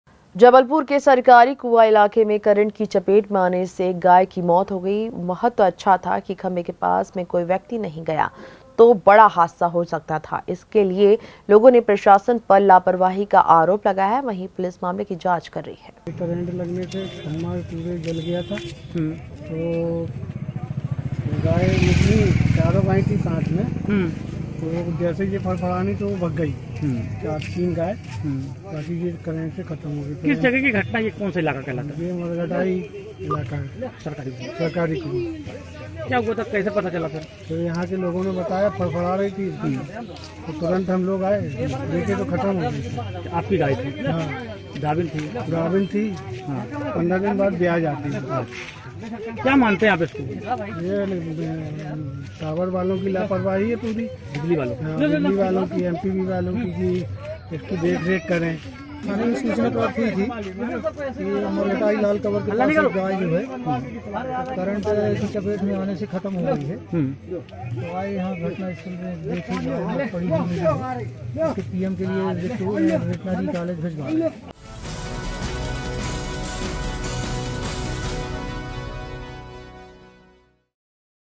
Jabalpur - करंट की चपेट में आने से युवक की मौत
जबलपुर के सरकारी कुआं इलाके में करंट की चपेट में आने से एक गाय की मौत हो गई। वह तो अच्छा था कि खंबे के पास में कोई व्यक्ति नहीं गया नहीं तो बड़ा हादसा हो सकता था। इलाके के लोगों ने प्रशासन पर लापरवाही का आरोप लगाया है वही पुलिस मामले की जांच कर रही है।